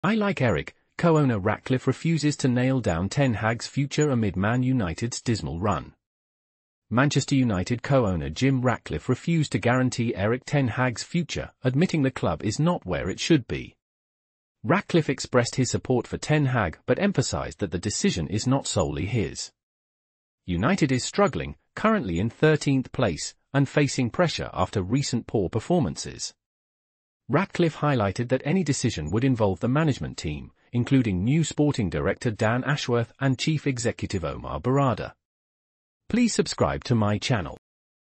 ‘I like Erik’: Co-owner Ratcliffe refuses to nail down Ten Hag’s future amid Man United’s dismal run
Manchester United co-owner Jim Ratcliffe refused to guarantee Erik ten Hag’s future, admitting the club is not “where it should be.” Ratcliffe expressed his support for Ten Hag but emphasized that the decision is not solely his. United is struggling, currently in 13th place, and facing pressure after recent poor performances. Ratcliffe highlighted that any decision would involve the management team, including new sporting director Dan Ashworth and chief executive Omar Berrada.